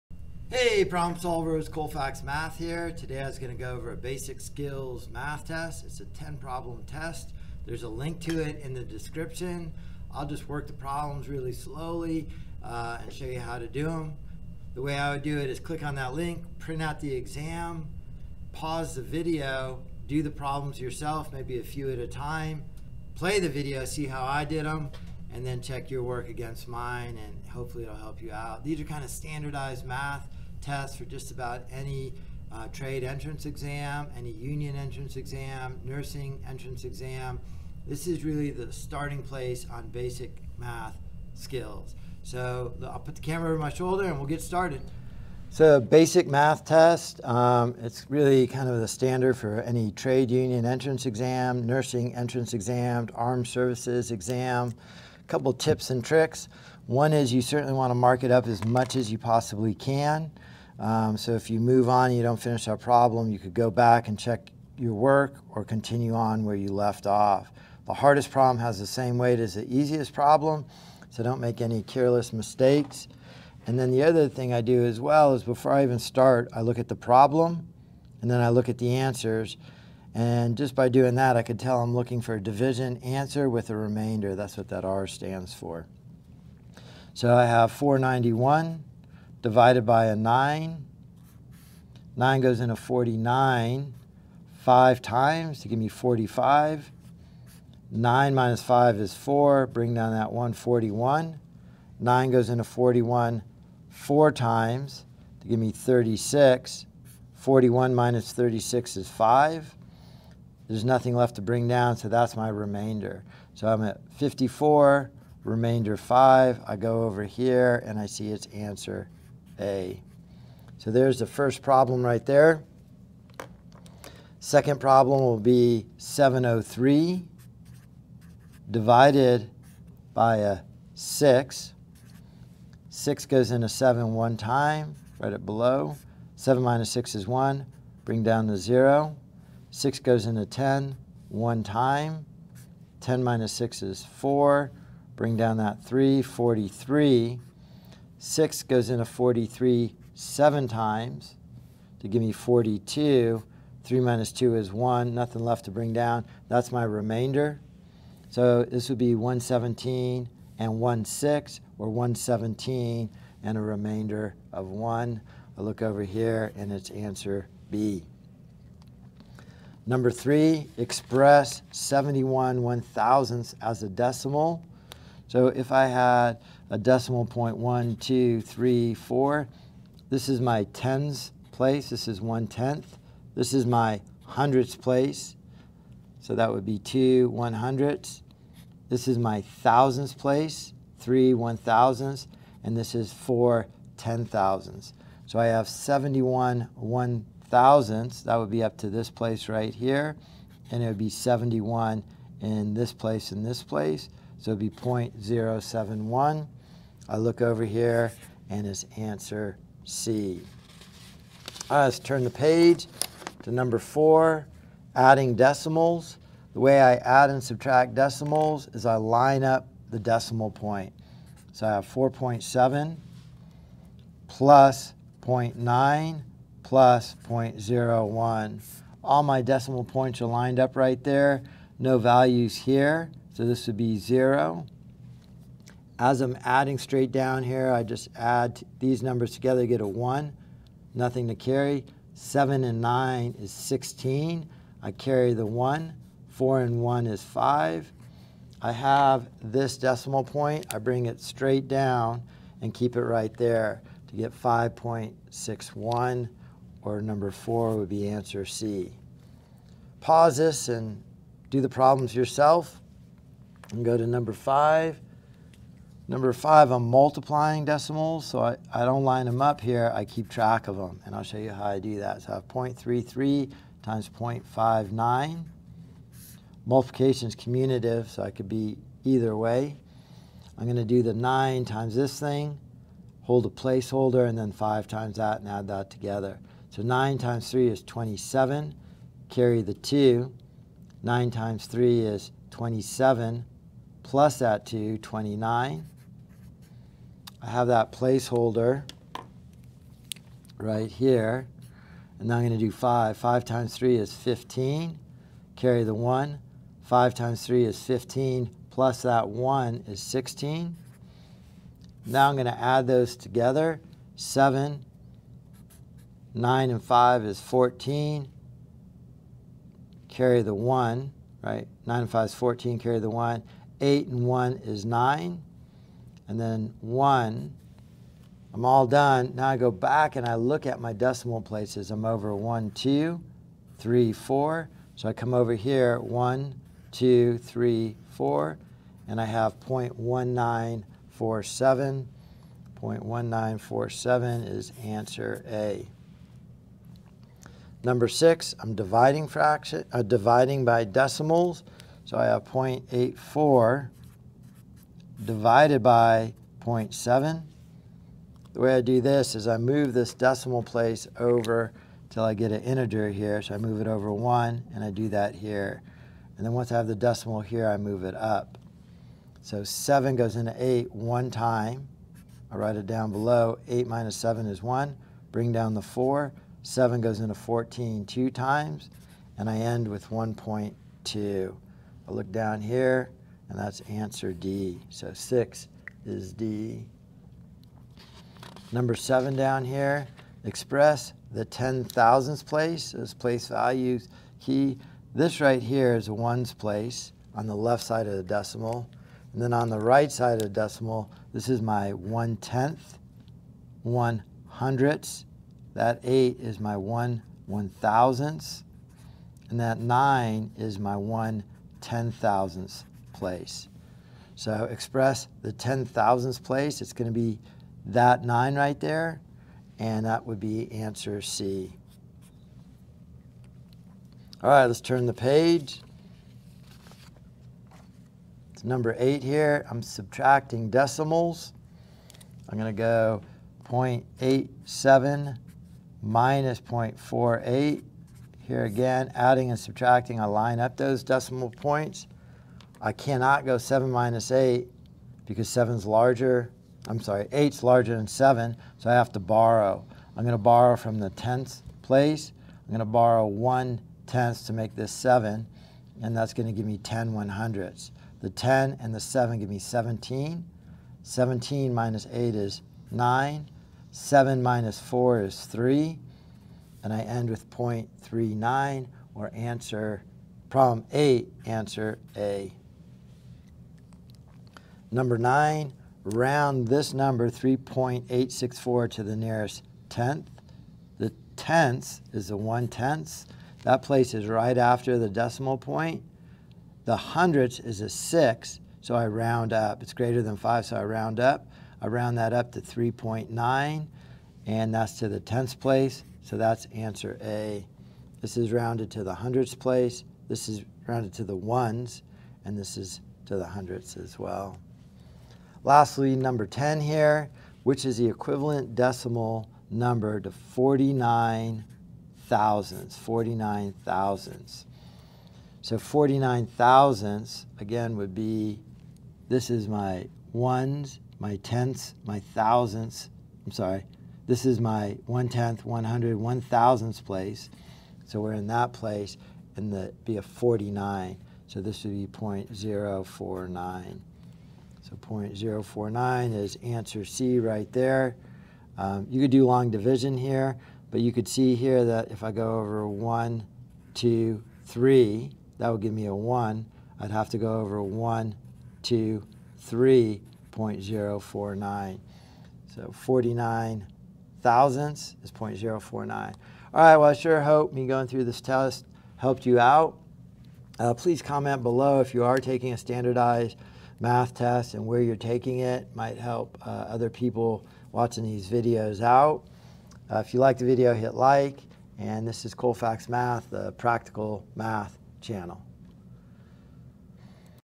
Free Basic Math Practice Test
Free basic math test with me doing the step-by-step solutions.  Good practice for any trade entrance exam, basic math for nursing entry, or military entrance. Tips and tricks for getting started on standardized math exams. Basic math test with link and solutions.